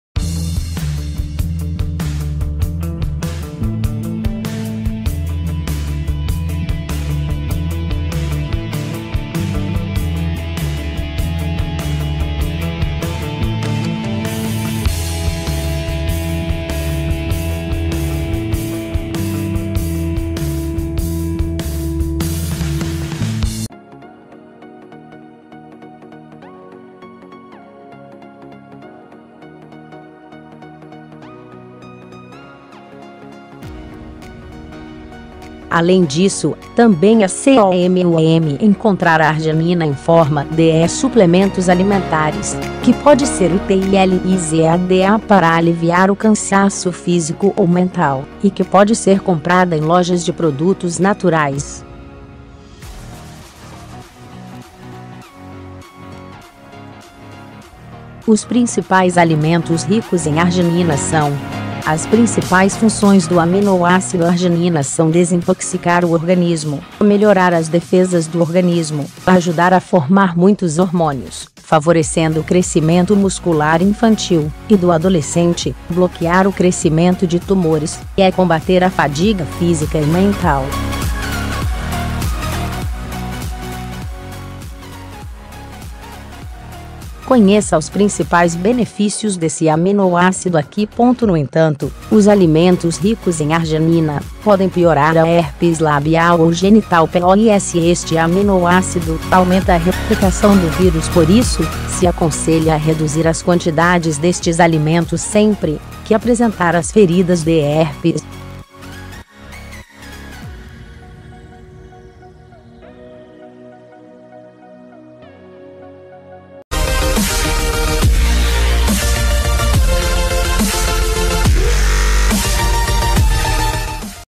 Saúde - Alimentos ricos em Arginina
Aarginina é um aminoácido não essencial e, assim como outros aminoácidos, está presente especialmente em alimentos ricos em proteínas, como queijo ou iogurte ou em outros alimentos como castanha-de-caju ou feijão, por exemplo.Além disso, também é comum encontrar a arginina em forma de suplementos alimentares, que pode ser utilizada para aliviar o cansaço físico ou mental e que pode ser comprada em lojas de produtos naturais.Os principais alimentos ricos em arginina são:  As principais funções do aminoácido arginina são desintoxicar o organismo, melhorar as defesas do organismo, ajudar a formar muitos hormônios, favorecendo o crescimento muscular infantil e do adolescente, bloquear o crescimento de tumores, e combater a fadiga física e mental.Conheça os principais benefícios desse aminoácido aqui.No entanto, os alimentos ricos em arginina podem piorar a herpes labial ou genital pois este aminoácido aumenta a replicação do vírus por isso se aconselha a reduzir as quantidades destes alimentos sempre que apresentar as feridas de herpes.